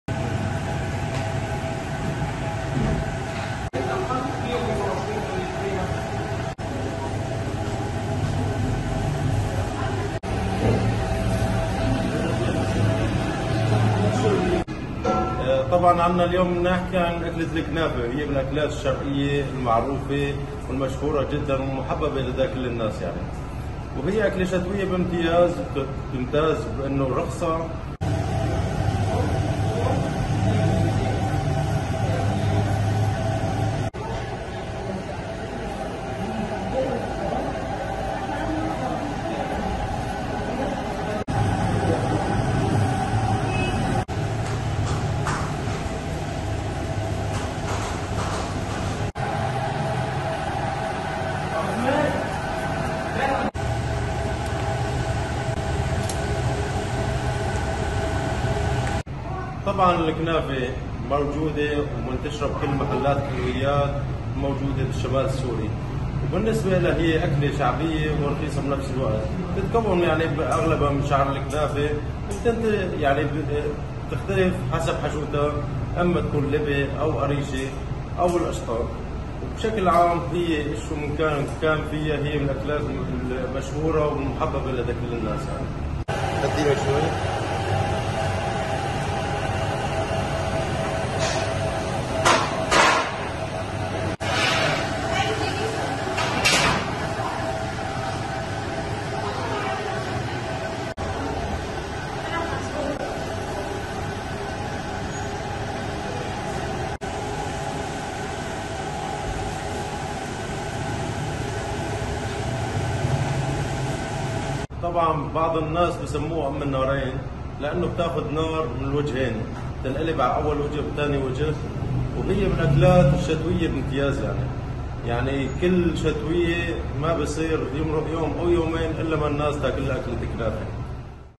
أبو الوليد يعمل على تحضير الكنافة في ريف إدلب والتي تعتبر من الحلويات الشتوية الأساسية لدى السوريين
تُعتبر الكنافة من الحلويات الشتوية الأساسية لدى السوريين، ومع دخول فصل الشتاء، يعمل الشاب ٫أبو الوليد" على تحضير الكنافة وجميع أصناف الحلويات، وتتميز الكنافة بكونها حلوى شهية ورخيصة الثمن، مما يجعلها محط إقبال من الأهالي في الريف الشمالي لمحافظة إدلب.